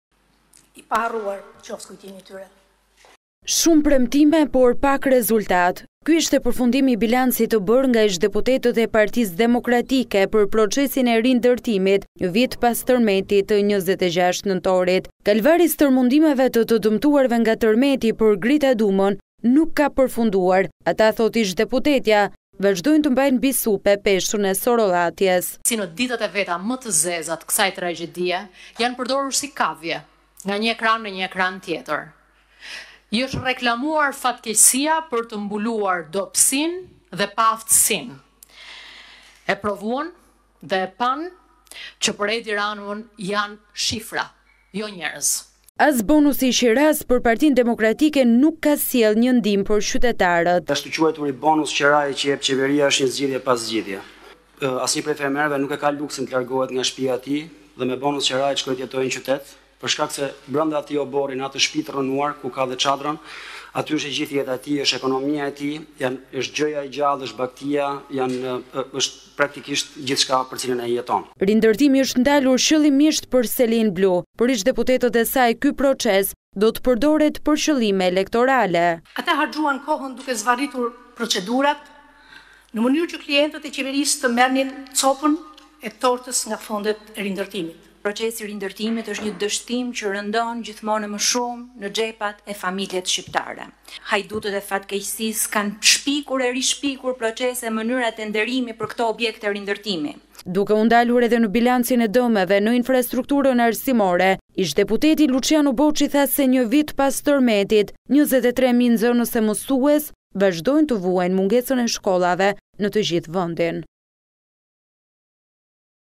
1 vit pas tërmetit, 'ekspertët' e PD 1 minutë heshtje! Bëjnë 'bilancin' e qeverisë për rindërtimin
Në fillim të kësaj konference është mbajtur një minutë heshtje, më pas deputetja Dhurata Çupi ka hedhur akuza ndaj qeverisë, e cila sipas saj ka harxhuar kohën duke zvarritur procedurat, për të përfituar nga fondet e rindërtimit. Sipas tyre PS gjatë këtij viti kanë bërë shumë premtime dhe pak rezultate. Grupi i PD që ka ndjekur procesin e rindërtimit thotë se qeveria po e përdor fatkeqësinë për të vjedhur dhe për zgjedhje.